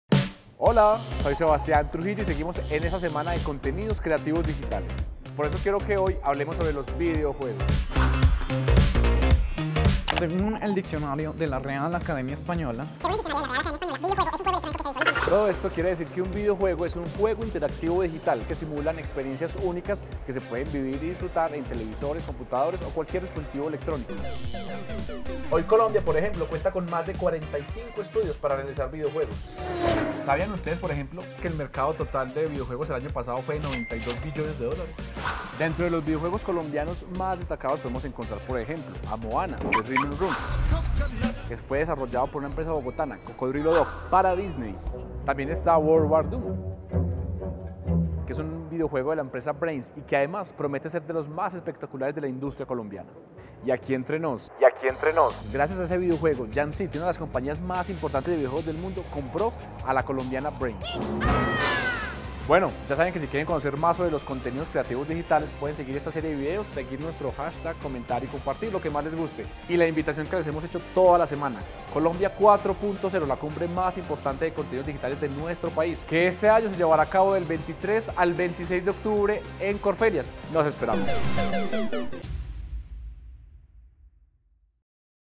Sumérgete en el mágico mundo de los videojuegos.
¿Te gustan los videojuegos 🎮 ? Detrás de lo que juegas hay toda una industria que cada día crece más, no te pierdas este videoblog donde te explicamos todo su proceso.👾